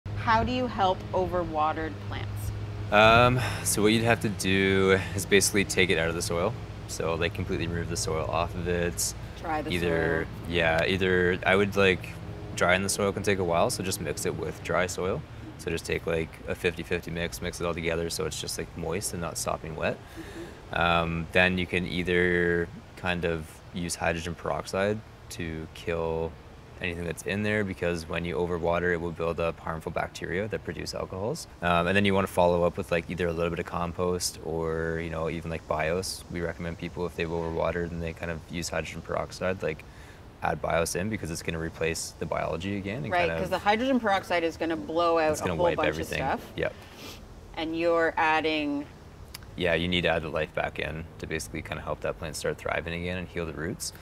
🪴💧How do you help overwatered plants? ➡️
Oh no! You’ve overwatered a much-loved plant and now it’s suffering! Don’t worry, Aaron is here to help:

🌿 First, remove the plant from the pot
🪴 Then, remove all the wet soil from the plant’s roots
🌿 It can take a long time for soil to dry, so instead, mix some dry soil with the wet in the pot to reduce the overall moisture level
🪴 If you’re worried about bad bacteria growing on your plant’s roots as a consequence of overwatering, use hydrogen peroxide or alcohol to kill the bad guys
🌿 But! Hydrogen peroxide will kill ALL the microbes (good and bad) in the soil
🍂 So — add compost or BIOS back in to put biology/life back into the soil
💗 This will help your plant to start to thrive again and feed its roots

For more houseplant care tips and tricks, make sure you check out the whole podcast episode, right